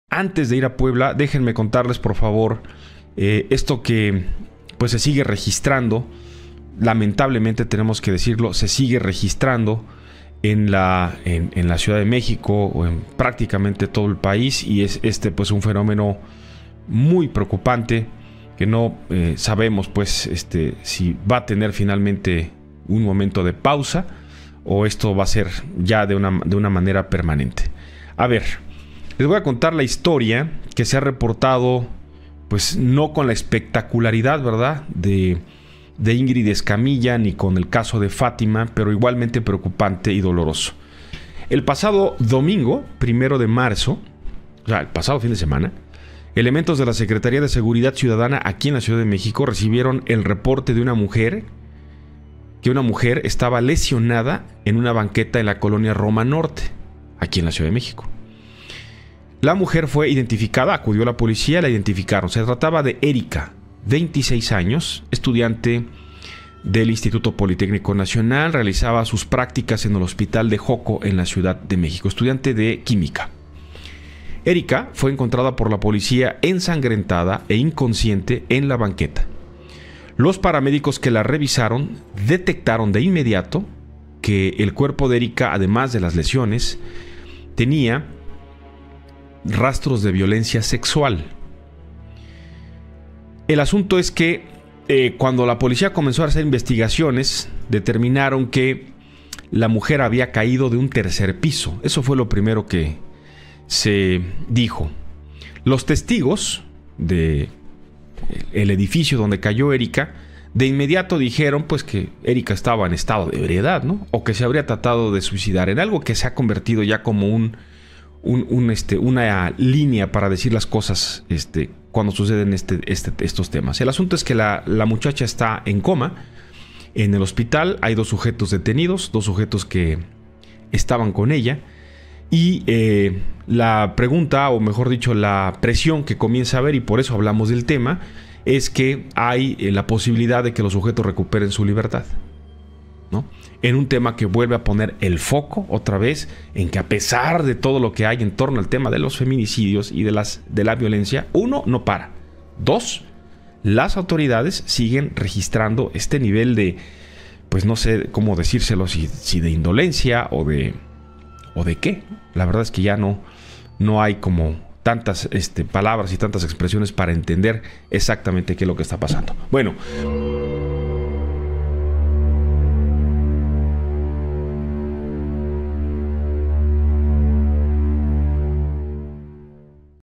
A Erika, estudiante del IPN la violaron y la lanzaron de un edificio en la CDMX
El primero de marzo, elementos de la Secretaría de Seguridad Ciudadana (SSC), recibieron el reporte de una mujer lesionada tendida en la banqueta en la colonia Roma Norte, ella fue identificada como Erika, de 26 años, quien realizaba sus prácticas en el Hospital de Xoco de la Ciudad de México (CDMX).